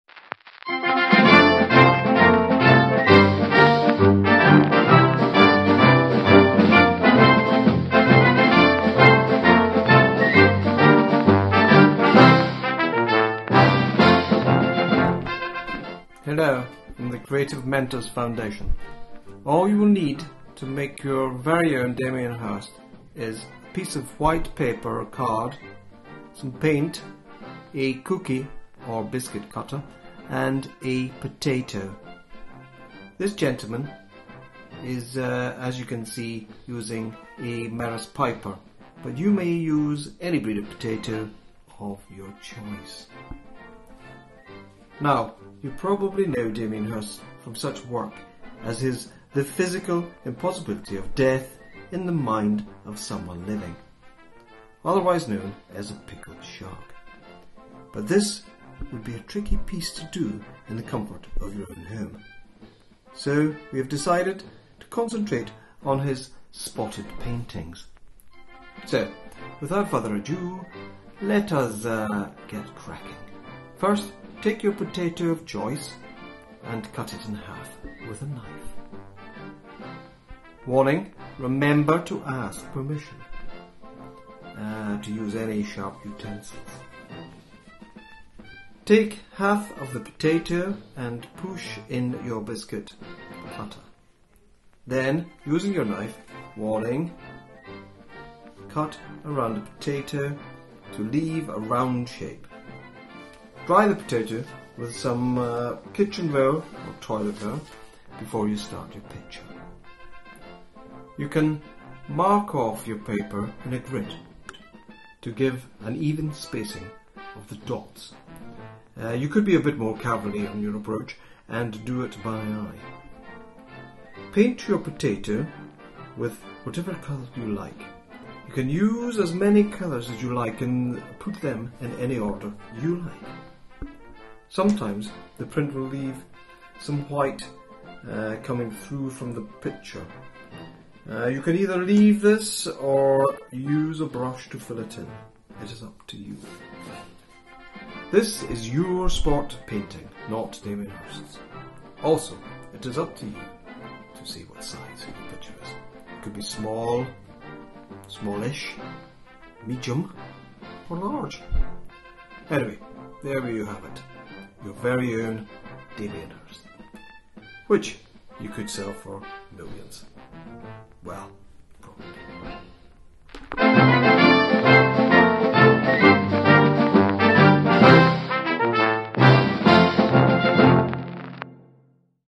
Creativity at Home • Damien Hirst inspired Dot Painting
"Potato Hirst"

Follow the steps with Stuart to create a Damien Hirst Dot style painting using just paint and potatoes. Aimed at primary and secondary students.